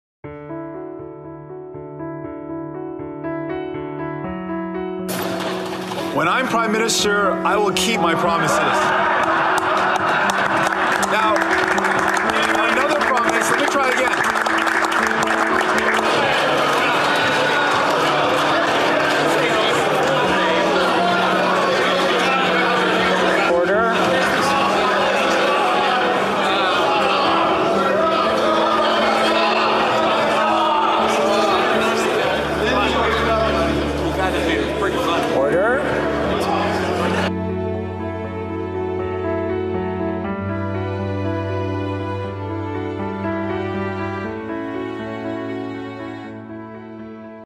Even Justin Trudeau laughs at Jagmeet Singh claiming when "I become Prime Minister"
That's got to sting to have Trudeau laugh at you.